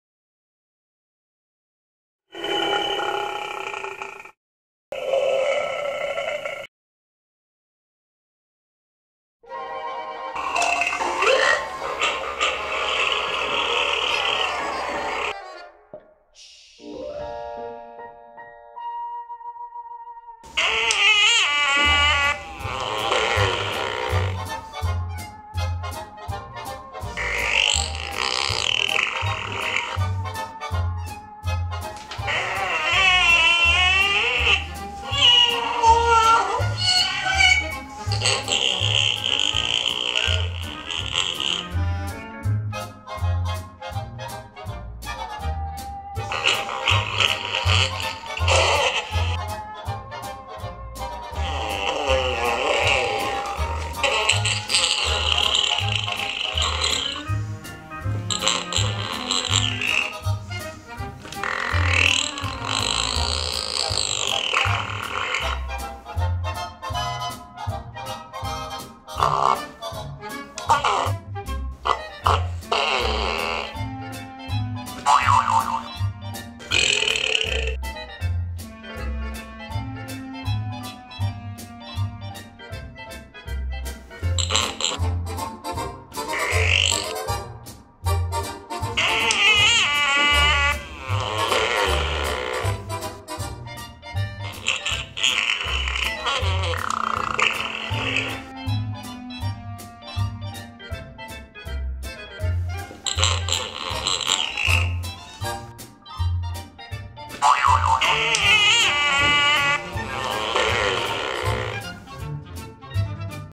The Ugglys Rank Pranks!
See what happens when an Uggly LETS LOOSE in a public bathroom!! Grrrross!